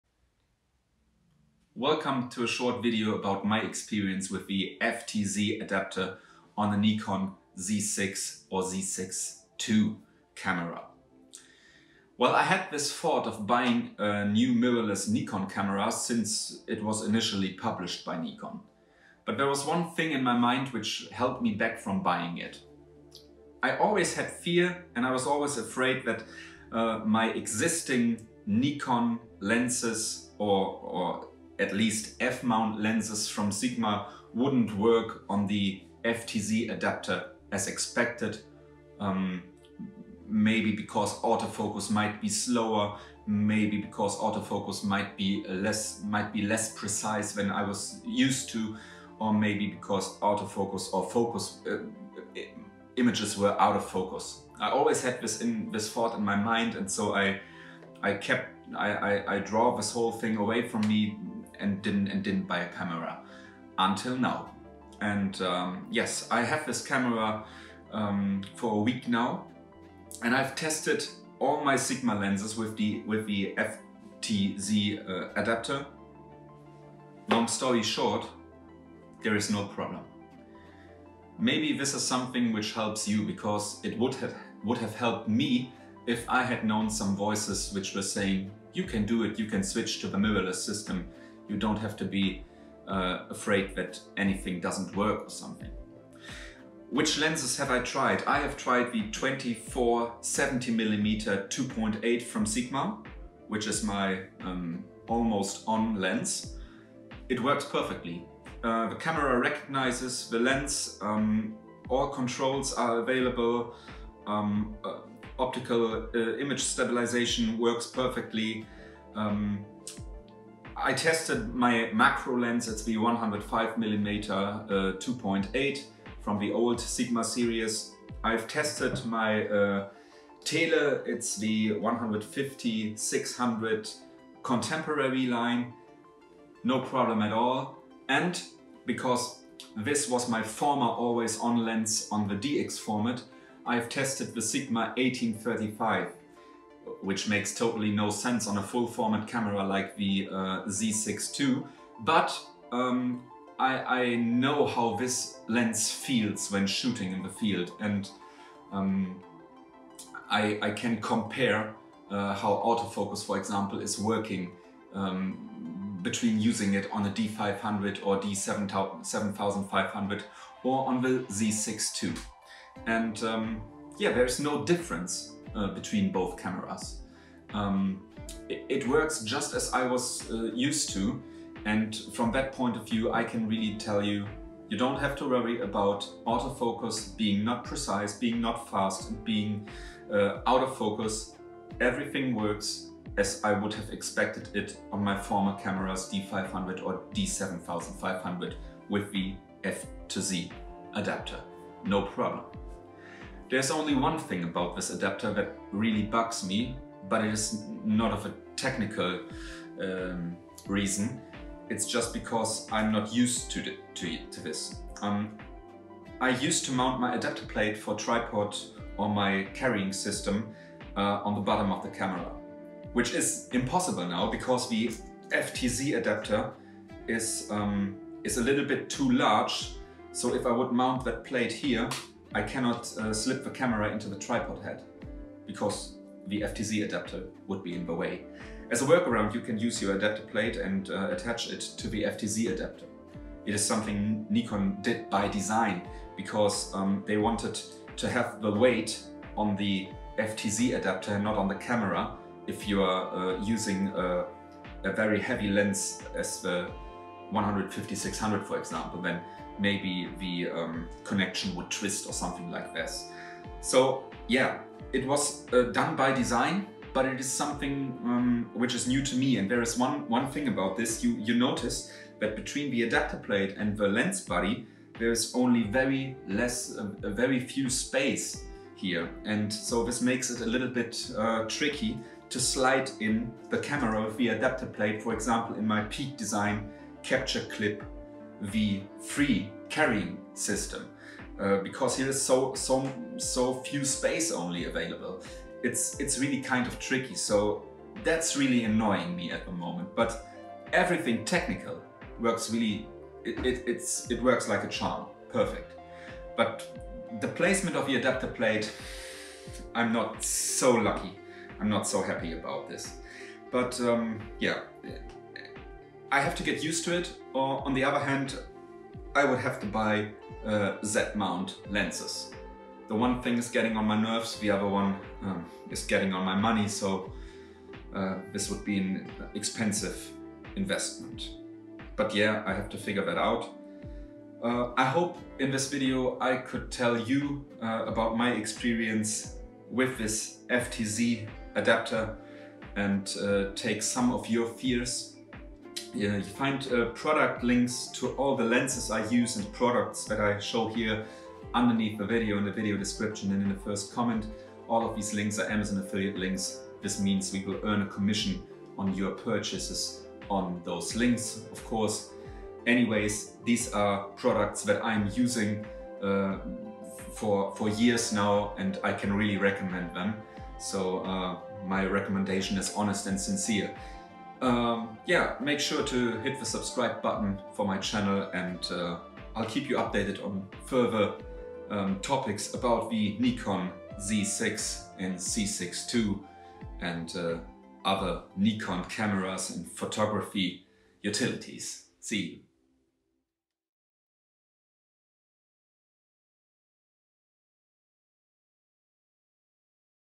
Review: Using Sigma F-Mount lenses with FTZ adapter on the Nikon Z6 II - does it work?
With the Z6 II I switched to full format and the Z-Mount. I wanted to continue using my favorite Sigma lenses with F-Mount and connect them to the mirrorless Nikon via FTZ adapter. In the run-up I had some concerns that the autofocus might be slower or less accurate. As it turned out this fear was unfounded, but one drawback remains – at least for me.